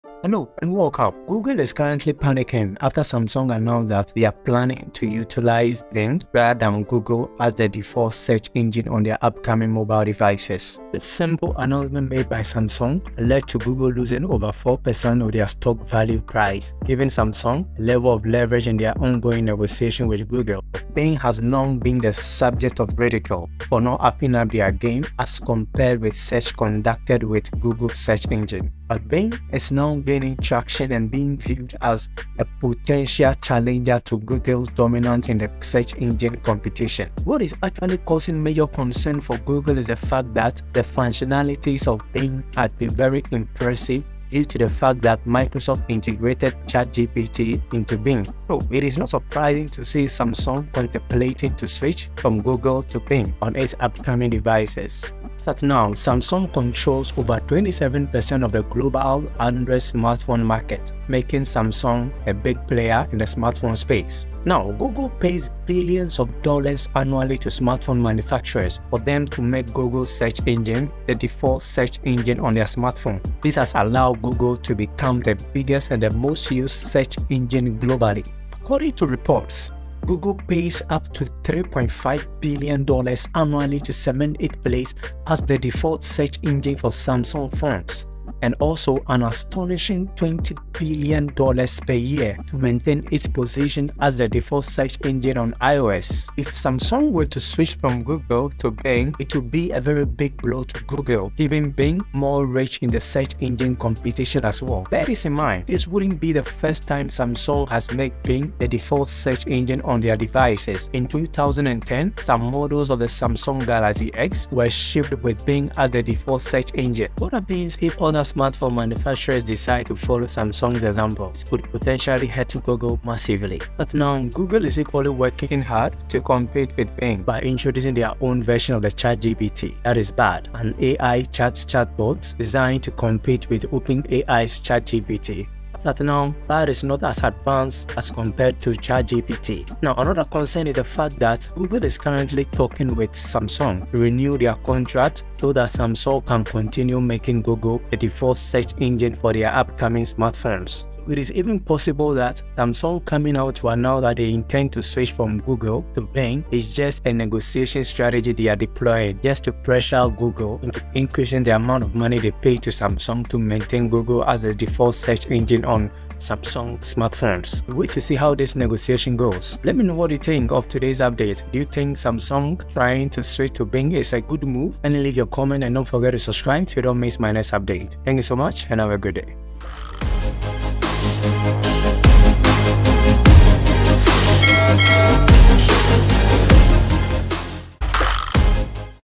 SAMSUNG plans to ditch Google For Bing , Google in Panic mood
Hello and welcome, Google is currently panicking after Samsung announce that they will be utilize Bing rather than Google as the default Search engine on their up coming mobile devices.
as you know  Bing had long been the subject of ridicule for not upping their game as compared with search conducted with Goole engine.,
but bing  it is now gaining traction and being viewed as a potential challenger to Google's dominance in the search engine competition.